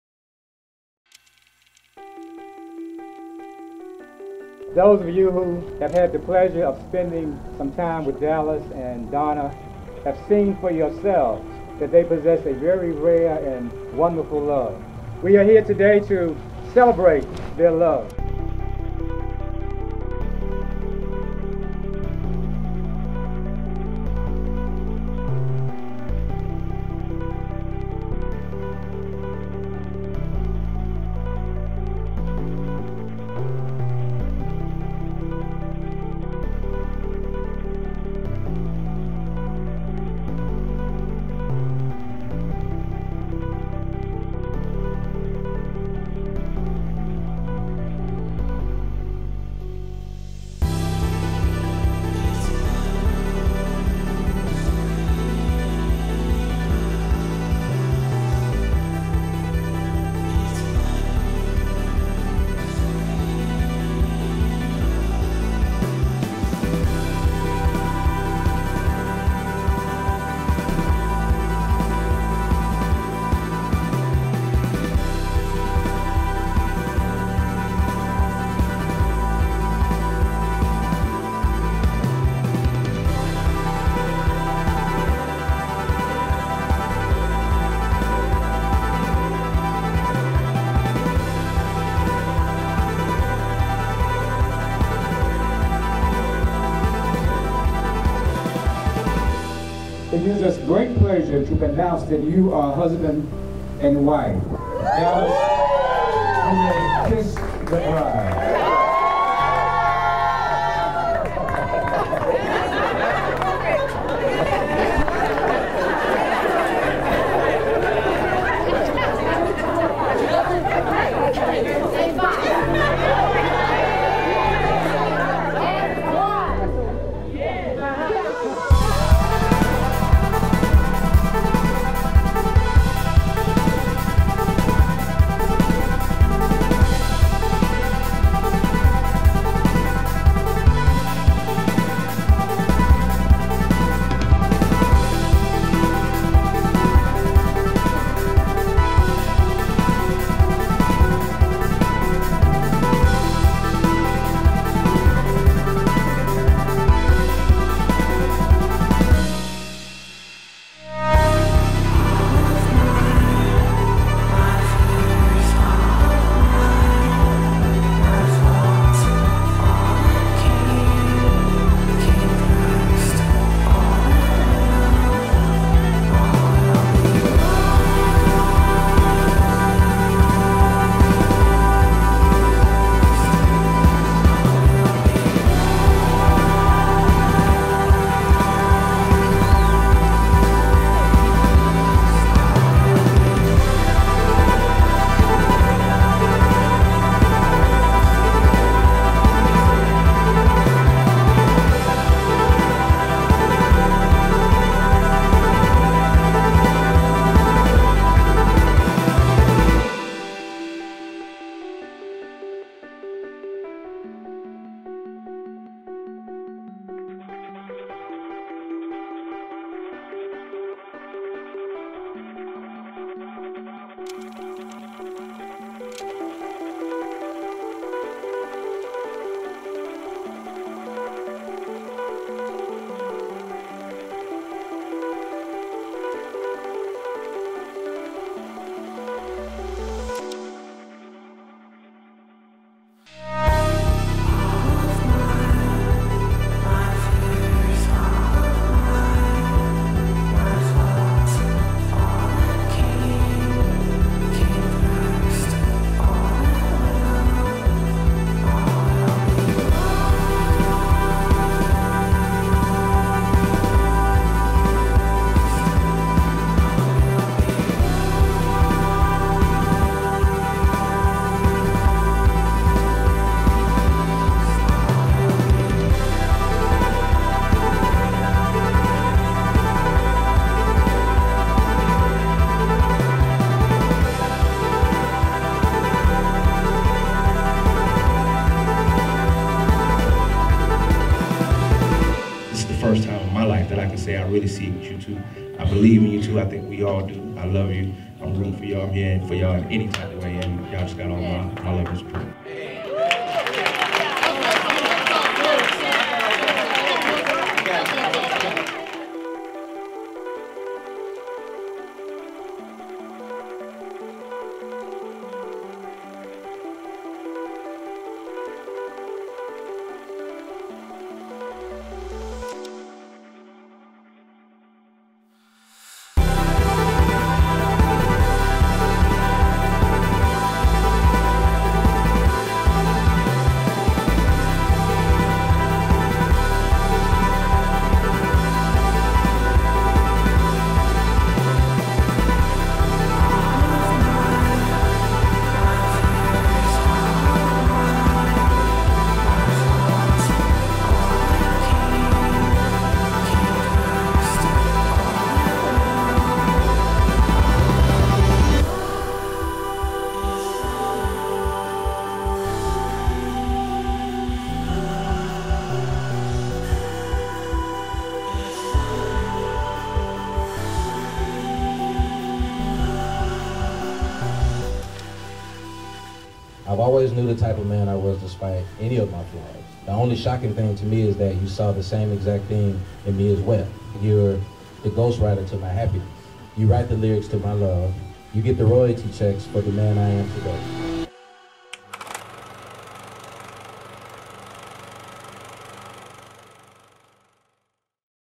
Harris Wedding 2016 Orlando
June 3, 2016 , The union of Dallis and Donna Harris at the Ocoee Lakeshore Center. Video by - Chiola Productions (Facebook)